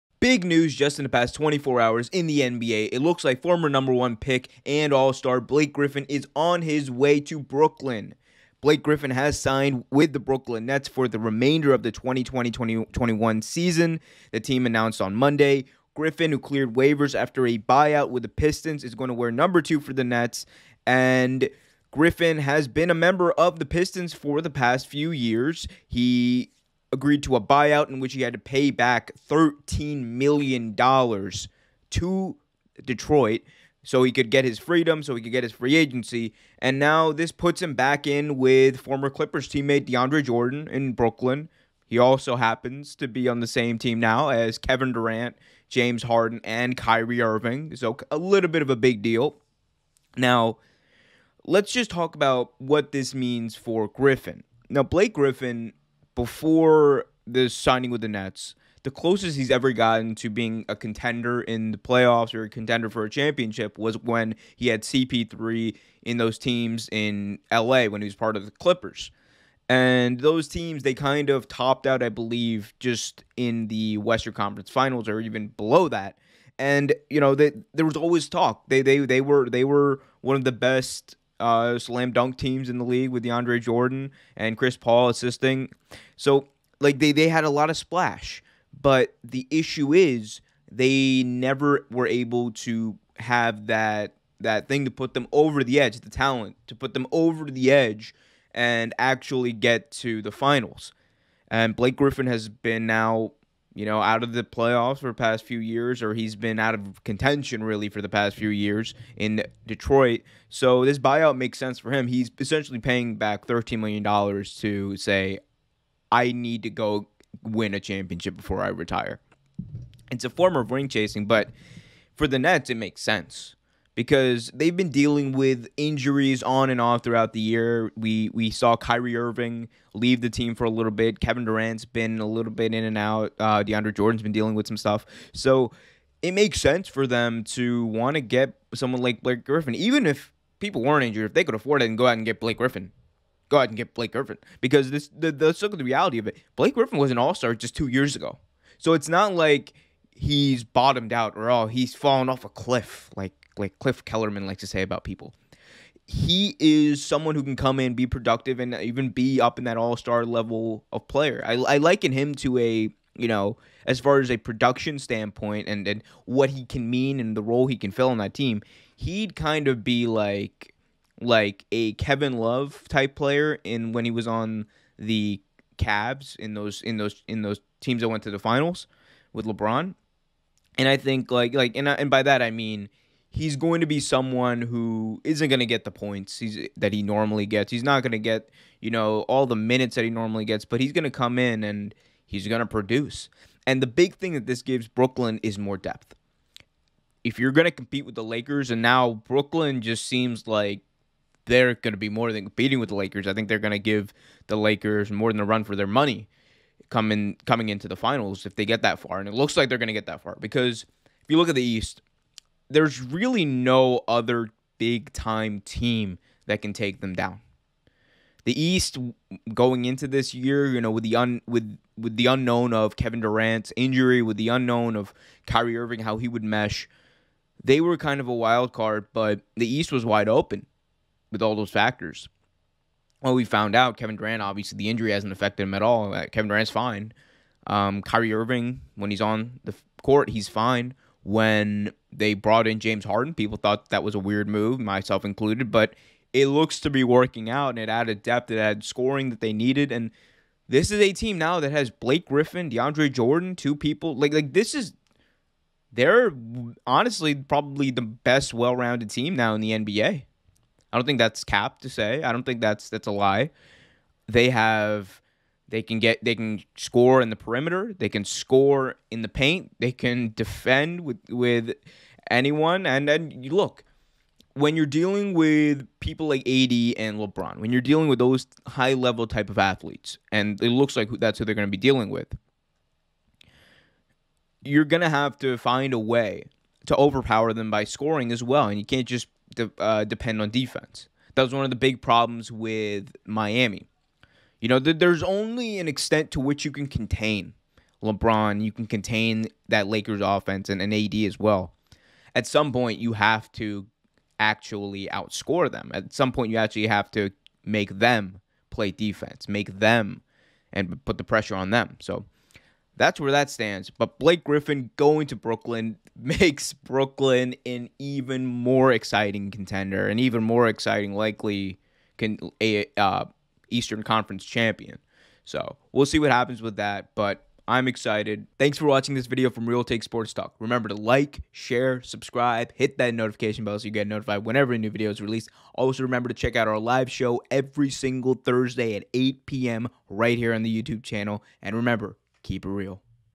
Blake Griffin SIGNS With the Brooklyn Nets
Former Clippers and Detroit Pistons All Star Blake Griffin has signed a deal with the Brooklyn Nets for the remainder of the 2021 season. He joins other all stars like Kyrie Irving, Kevin Durant, James Harden, and former Clippers teammate Deandre Jordan in Brooklyn.